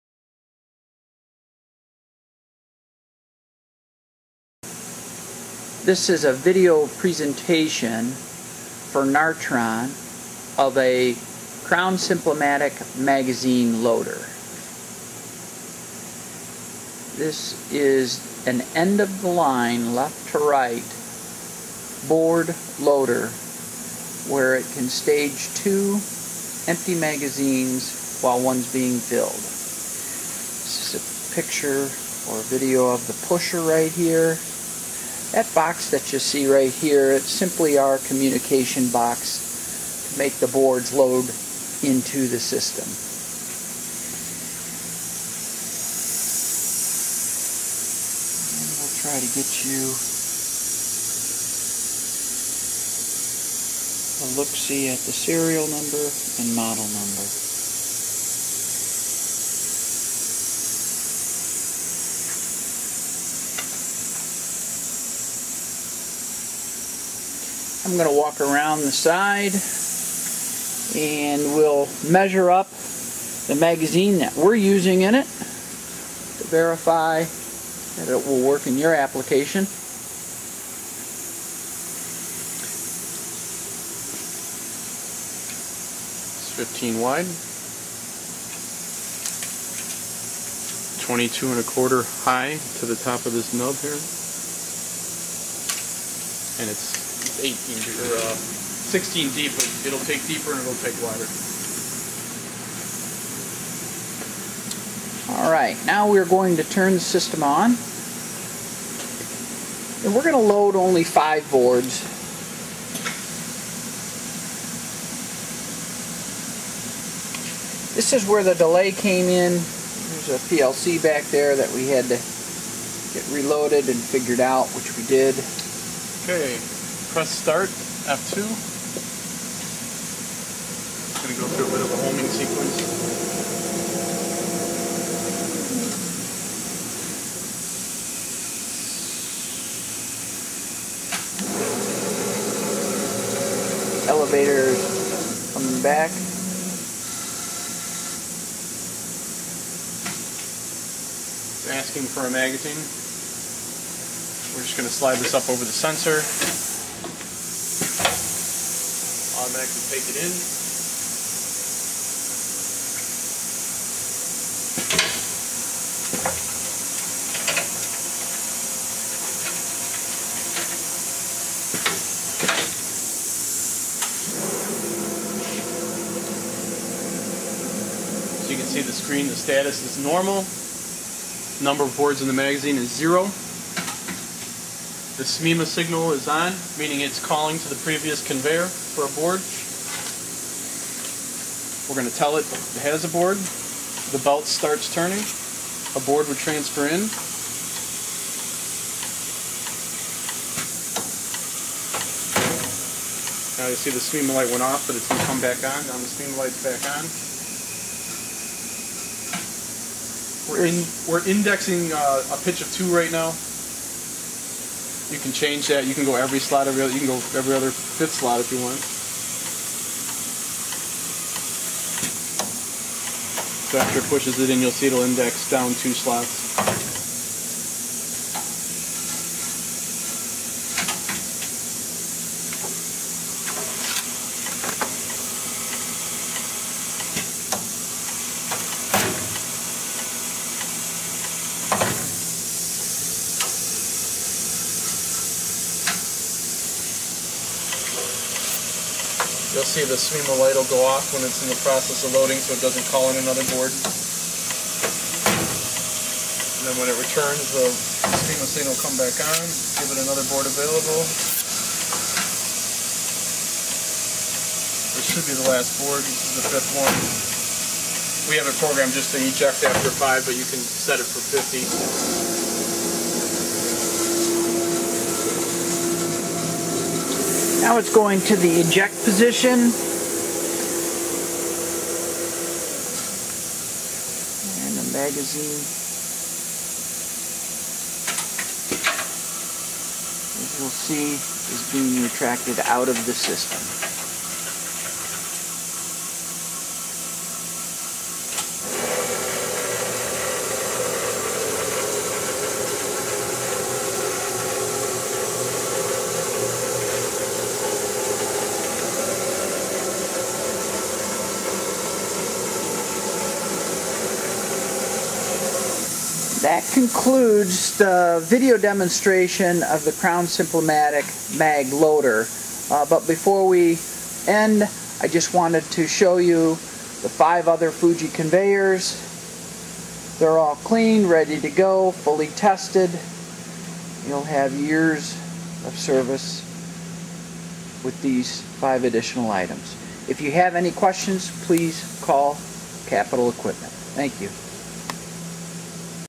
Simplimatic PCB Magazine Handler for end of line
This is a customer demonstration of fully functionality for a Simplimatic magazine handler, which collects pcbs at the end of a smt line.  Collected pcbs are loaded into magazines for removal.